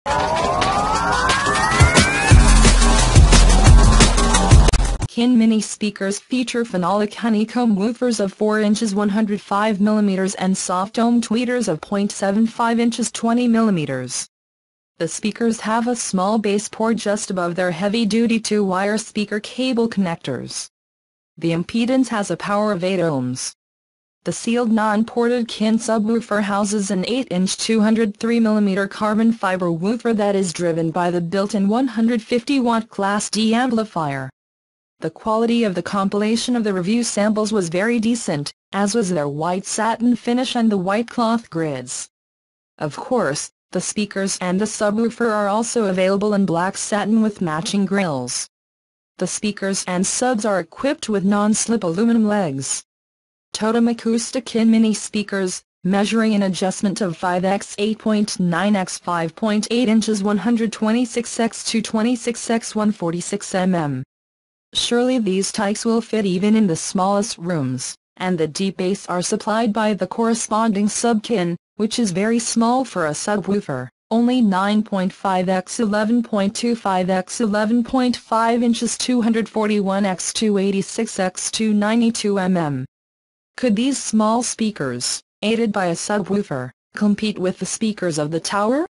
The mini speakers and KIN Sub a small and sweet three-piece system for music and movies.
KIN Mini Speakers feature phenolic honeycomb woofers of 4 inches 105 mm and soft dome tweeters of .75 inches 20 mm. The speakers have a small bass port just above their heavy-duty two-wire speaker cable connectors. The impedance has a power of 8 ohms.

The sealed non-ported KIN subwoofer houses an 8-inch 203-mm carbon fiber woofer that is driven by the built-in 150-watt class D amplifier. The quality of the compilation of the review samples was very decent, as was their white satin finish and the white cloth grids. Of course, the speakers and the subwoofer are also available in black satin with matching grilles. The speakers and subs are equipped with non-slip aluminum legs.

Totem Acoustic KIN Mini speakers, measuring an adjustment of 5 x 8.9 x 5.8 inches 126x226x146 mm. Surely these tykes will fit even in the smallest rooms, and the deep bass are supplied by the corresponding Sub KIN, which is very small for a subwoofer, only 9.5 x 11.25 x 11.5 inches 241x286x292 mm. Could these small speakers, aided by a subwoofer, compete with the speakers of the tower?